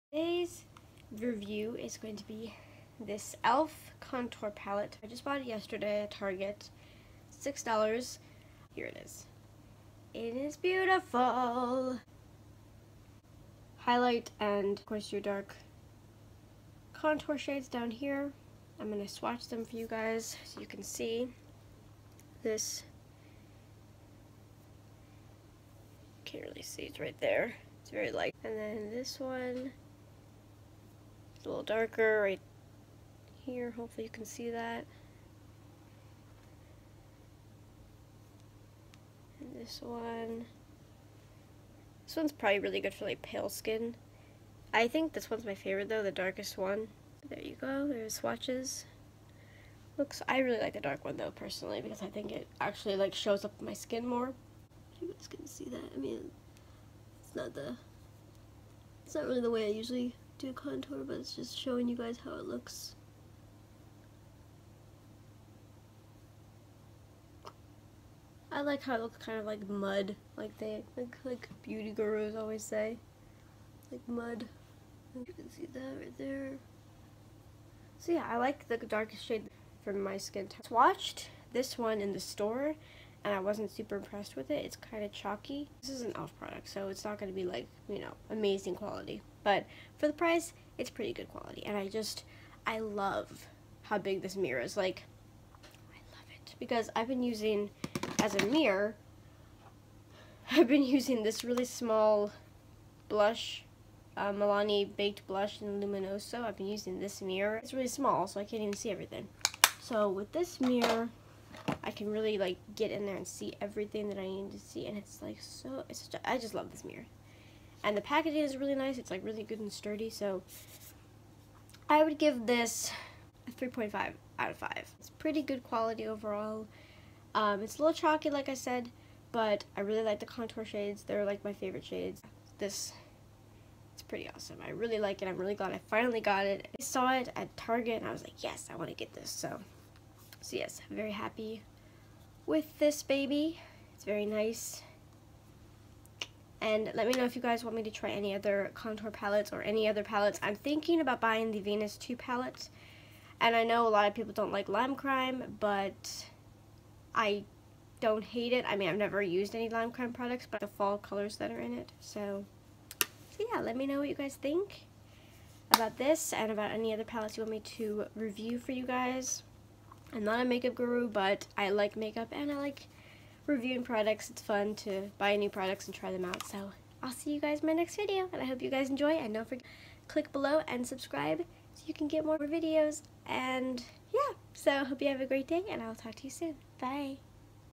ELF CONTOUR PALETTE | REVIEW
Hey guys! I really like makeup and I like revising new products! So here is an Elf review, I hope you like it!